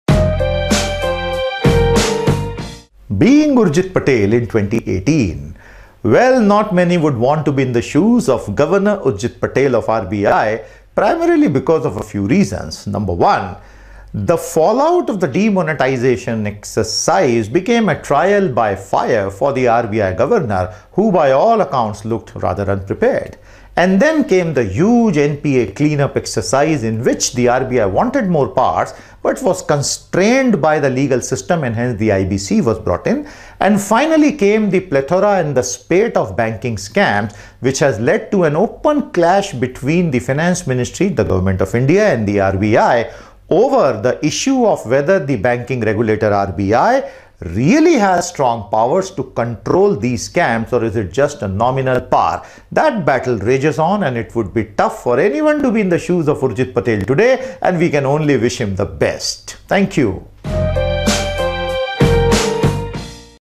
60 Seconds # 11 : Being Urjit Patel in 2018!
60 Seconds # 11 : Being Urjit Patel in 2018!

Understand a topic's gist in less than 1 minute, with our 60 Seconds series. Quick, effective and sharp!